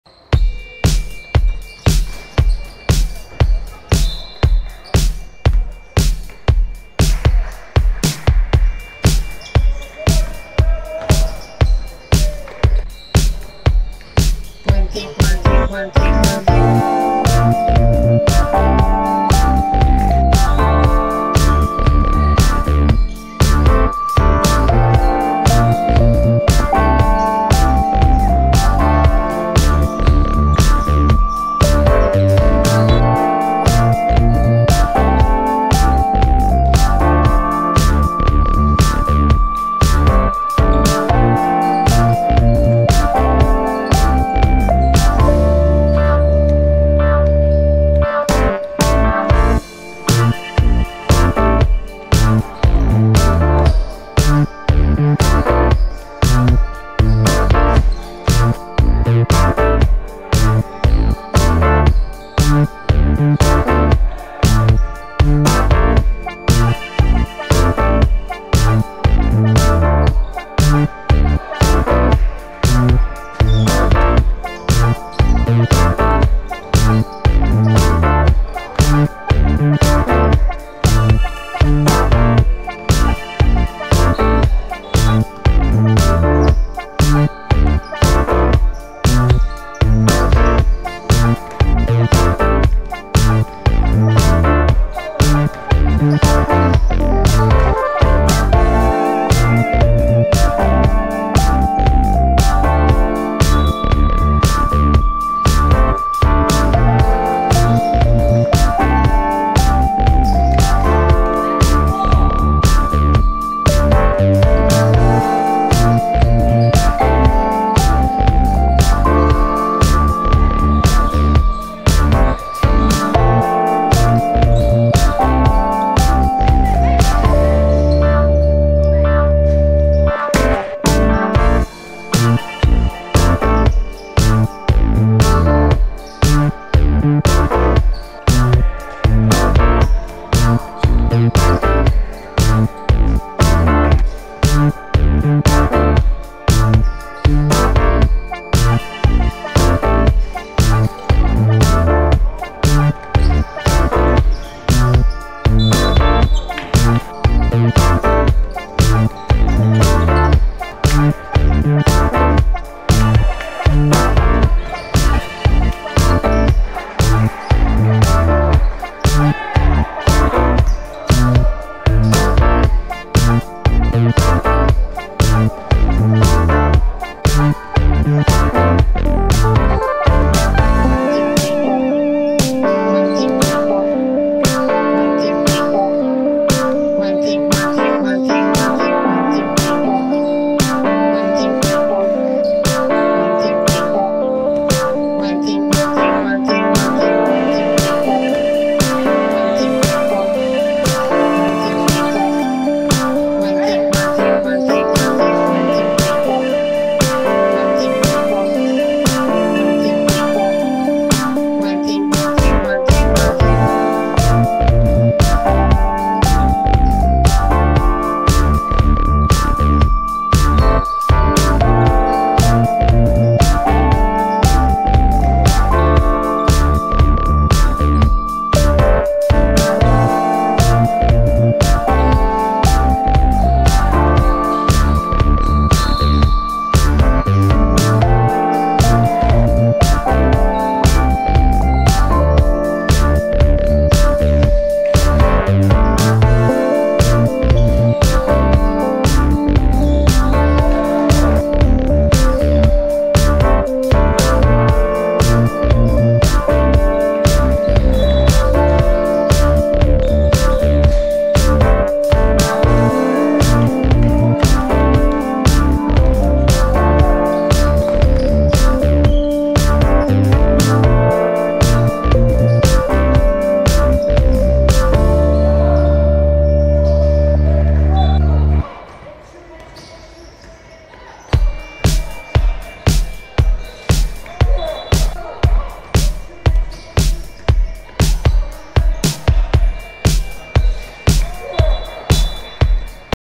03 12 Tigers vs Wildcats – Prospect Div Jesień 2022
Tigers 69:71 Wildcats
A.Kozłowicz 18pkt 19zb 8ast 4stl
D.Kazimierczak 31pkt 4x3 3ast 5stl